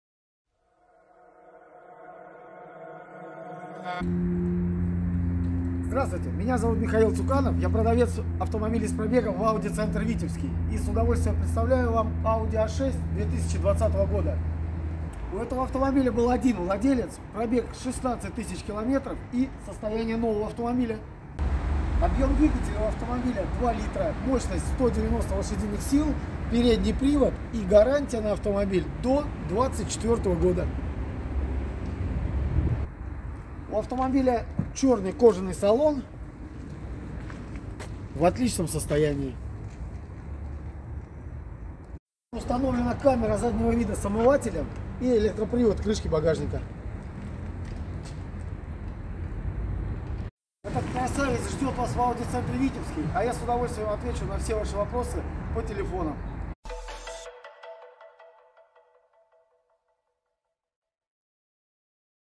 Audi A6 2.0 c пробегом Инчкейп
В продаже автомобиль с пробегом Audi A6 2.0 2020 г.в.